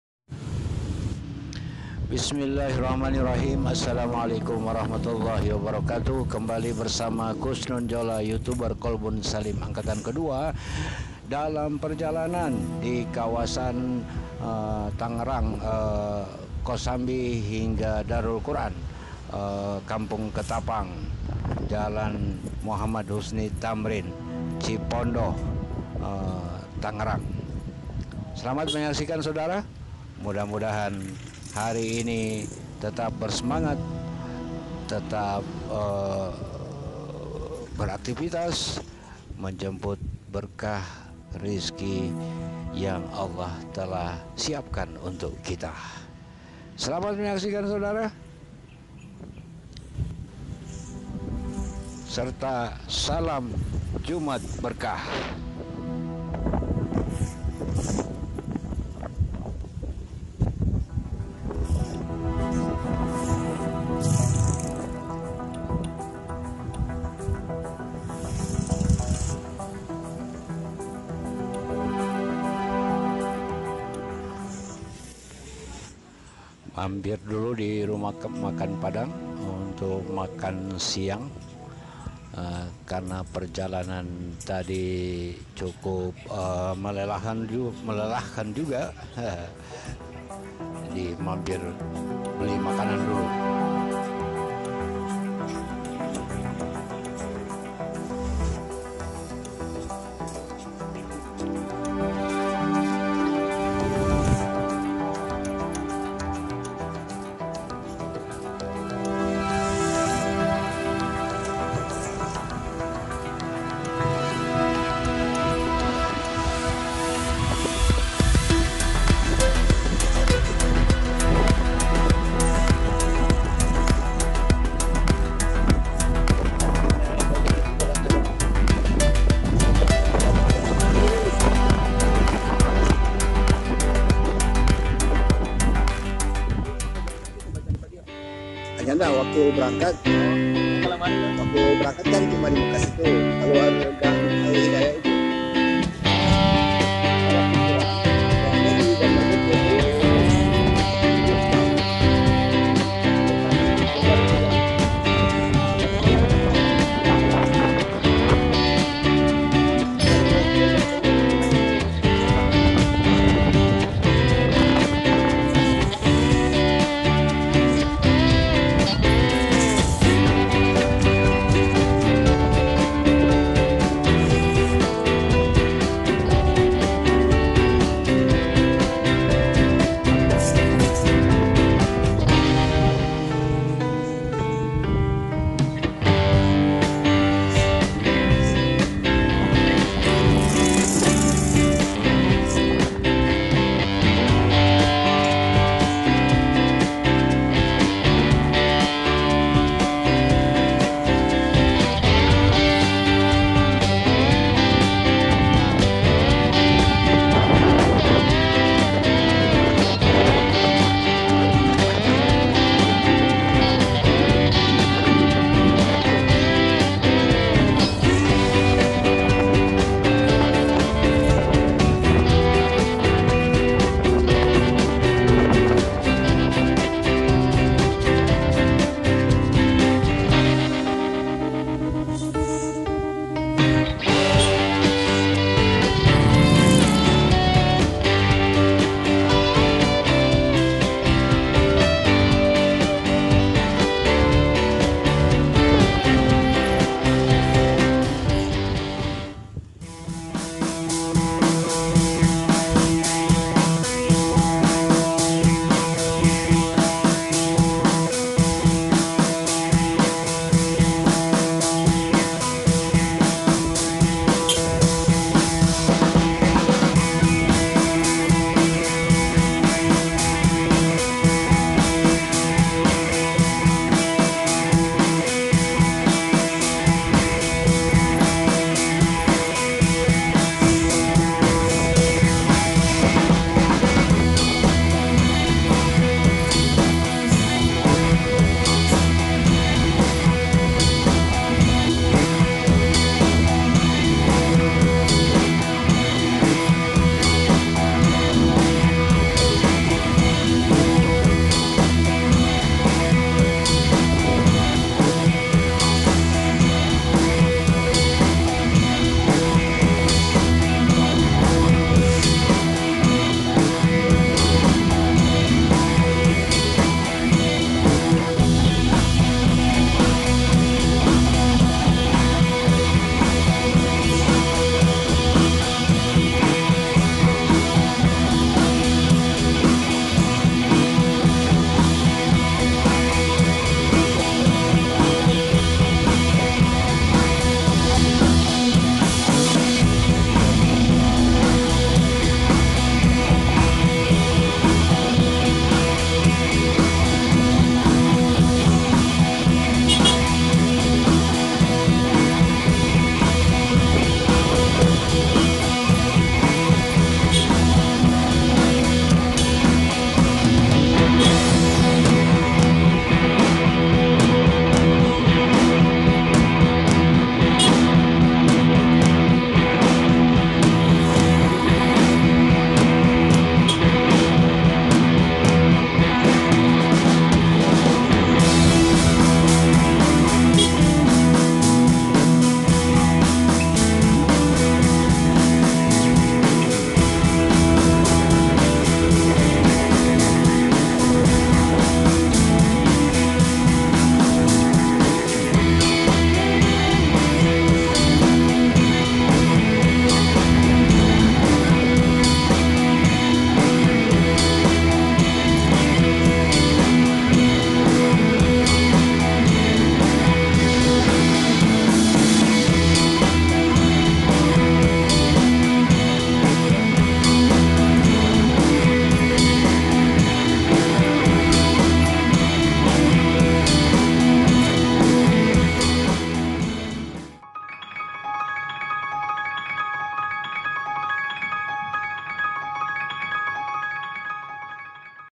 #162. PERJALANAN Jadi Saksi.
Solo Touring. Perjalanan ini dari KOSAMBI hingga Ketapang Cipondoh Tangerang BANTEN.